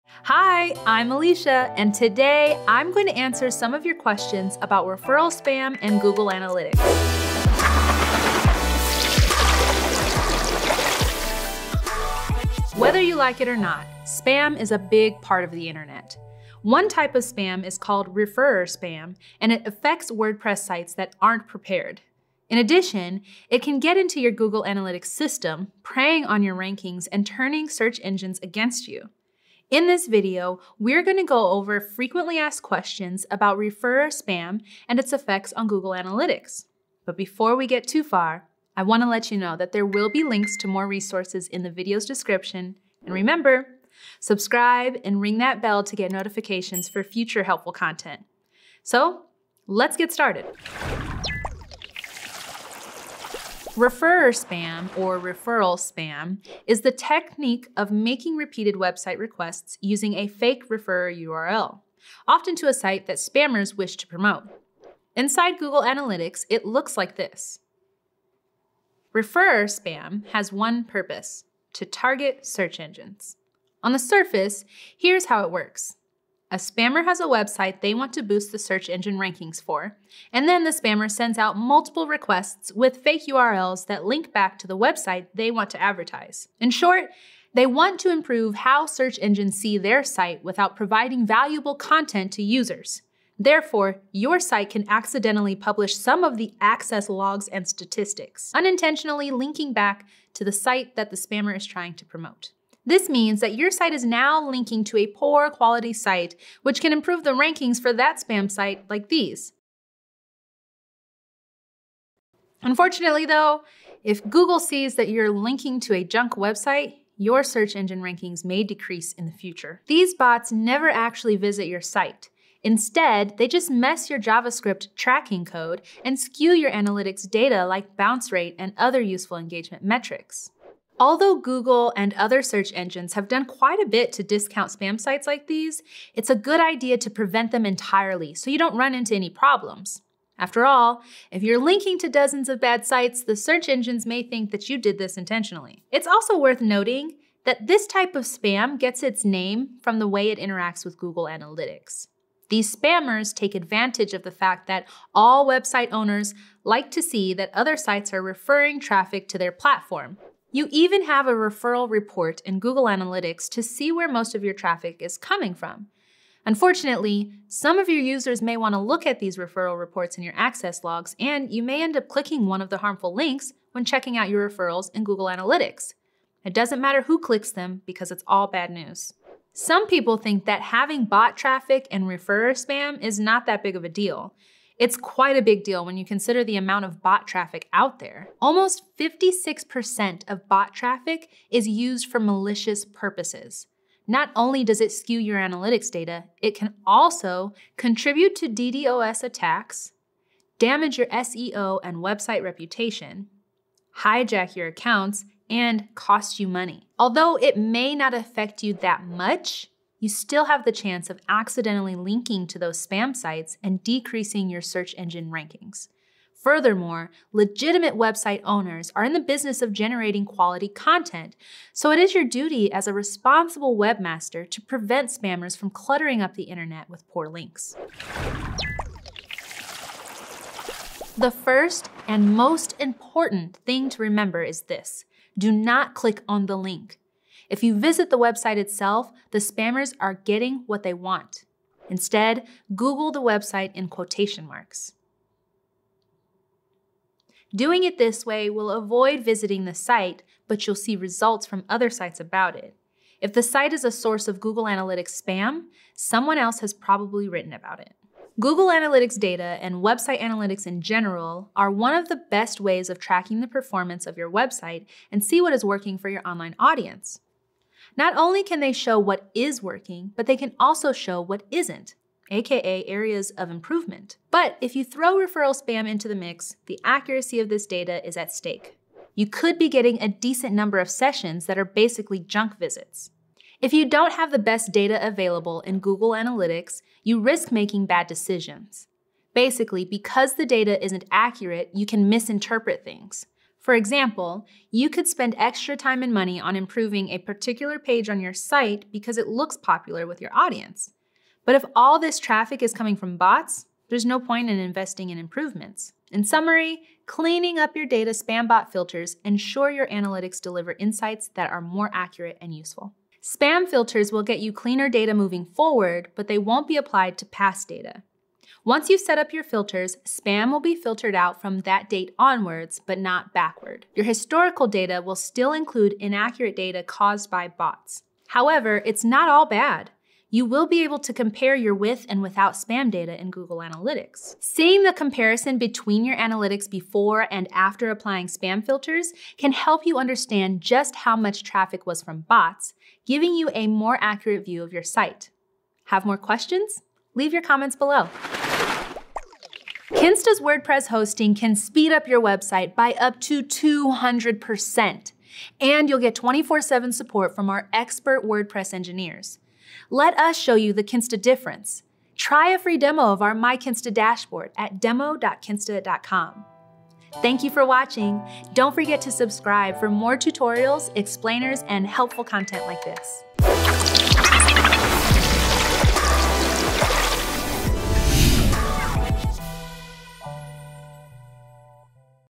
What is Referral Spam in Google Analytics
Whether you like it or not, spam is a big part of the internet. Some types of spam are more interested in using your server space to send out requests, while some take advantage of users to send over harmful malware and viruses.

One type of spam is called referrer spam and it affects WordPress sites that aren’t prepared. In addition, it can get into your Google Analytics system, preying on your rankings and turning search engines against you.

Tune in to learn more about referral spam, and how to deal with them in Google Analytics!

🕘Timestamps

0:00 Intro
0:52 What Is Referrer (Or Referral) Spam?
2:29 Why Is It Called Referral Spam?
3:01 What Are the Benefits of Blocking Bot Traffic?
3:49 Can I Visit the Spam Sites Found in Google Analytics?
4:19 Why Is Filtering Spam from My Google Analytics Important?
5:16 Can I Clean up Historical Google Analytics Data?

📚Resources

📙 Full Guide on How to Block Referrer Spam in Google Analytics